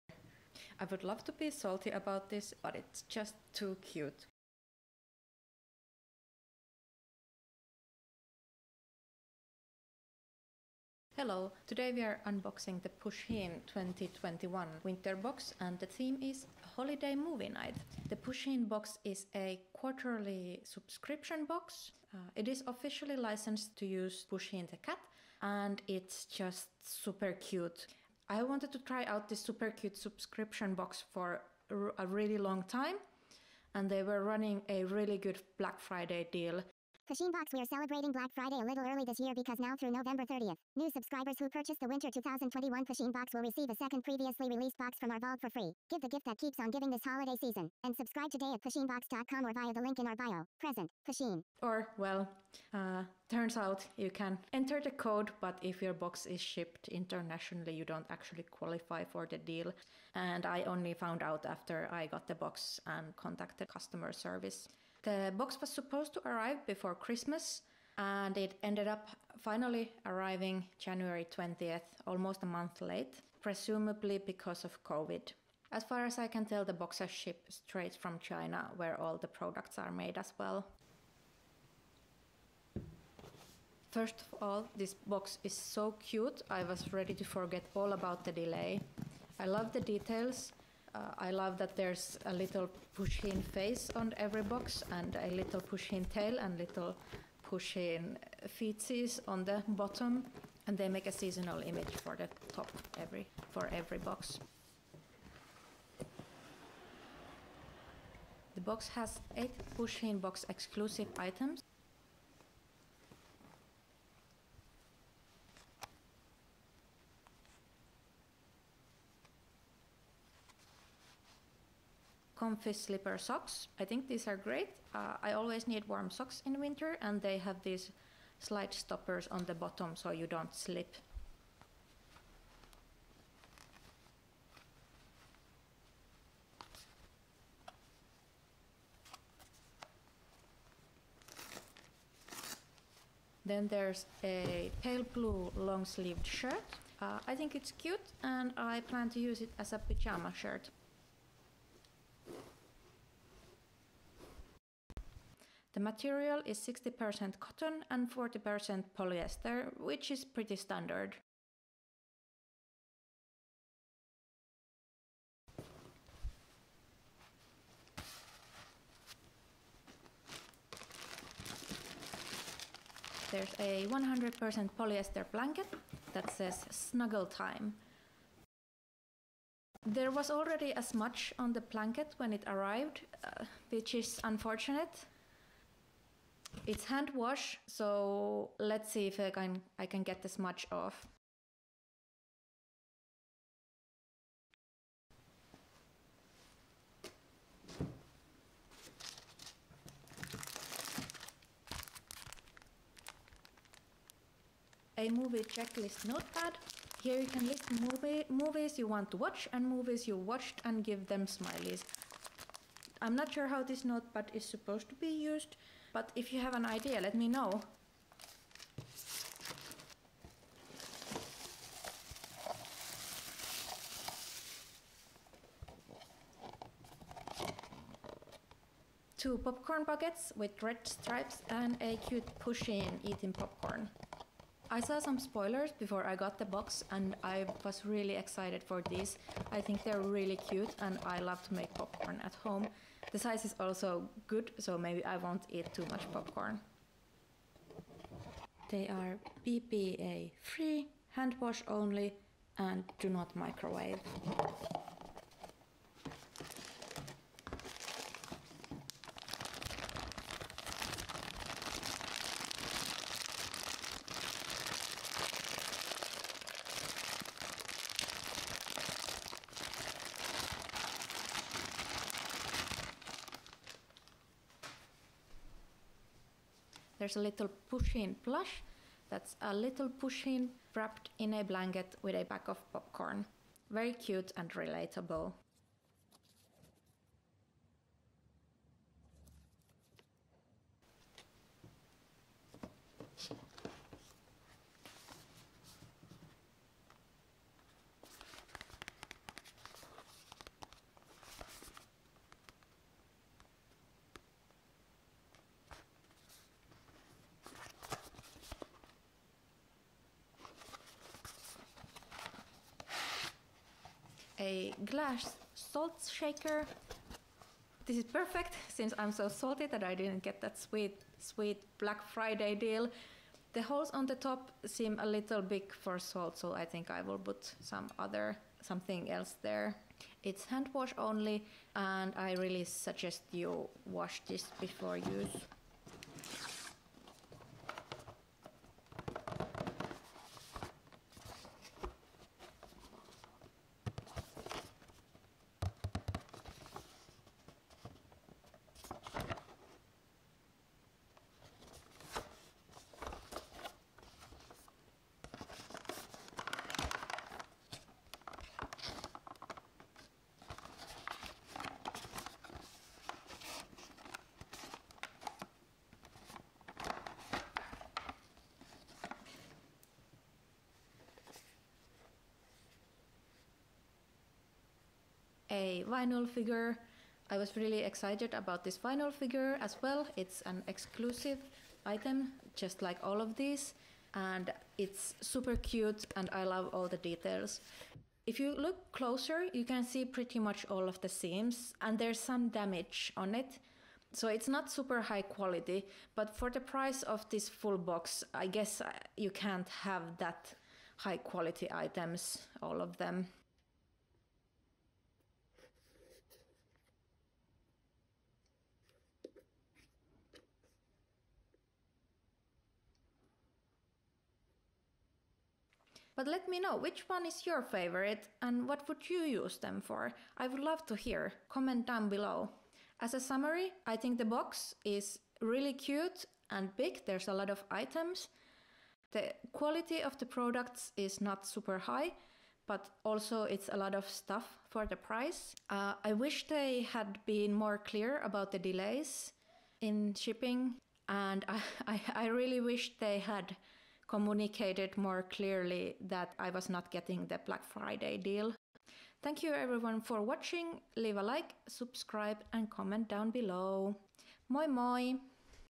Pusheen Box unboxing and honest review: Winter "Holiday Movie Night" 2021
After delays I finally got my long-awaited Pusheen box!
In this video I want to tell you about my experience and show you close ups of the items so you can decide are they worth it for you.
Let me know if you have any other questions

Here is what is included:
Intro, experience and shipping: 0:10
Winter Pusheen Box 1:20
Comfy slipper socks 1:52
Pusheen Pullover Shirt 2:21
Cuddly sherpa-lined blanket 2:51
Movie Checklist Notepad 3:20
Two Popcorn buckets 3:39
Pusheen plush 4:18
A glass Salt shaker 4:55
Cozy Pusheen Vinyl figure 5:37
Summary and final thoughts 7:00

What is Pusheen Box?
Pusheen Box is a quarterly subscription box, licensed to use Pusheen the cat.
You get 8 Pusheen Box exclusive items

When is Pusheen box shipping?
My Pusheen box shipped almost 2 months after ordering it. Some people got it before Christmas. Delays are presumably caused by covid.
Boxes ship straight from china where all the products are made as well.

Is Pusheen Box worth it?
You get 8 cute items and a cute box. However, the items are not super high quality and some were even dirty or damaged upon arrival.
For international (non-US) customers the shipping and taxes can almost double the price.
Take into account that the package is not declared before shipping and you need to handle the customs declaration yourself.